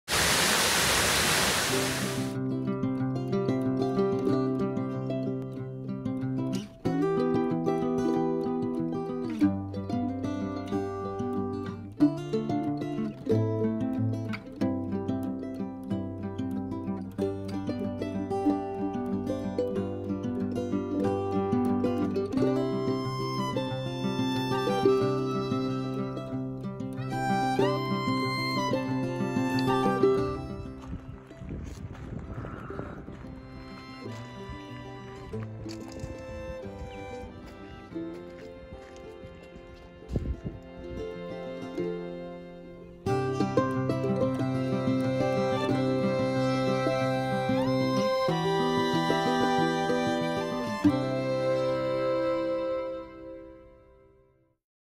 Snaefellsnes drive to GOT filming site.
We drove throw snow and hail..to see this beautiful mountain. Kirkjufell.